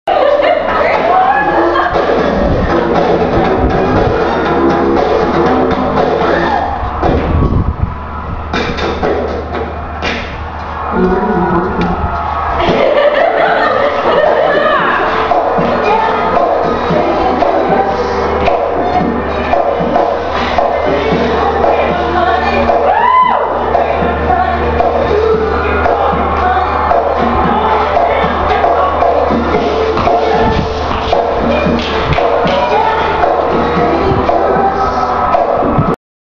Todd MJ Dancing (Rotoscoped)
Todd dancing to the King of Music. I decided to see how it would look if I "rotoscoped" this short piece using Photoshop.and After Effects.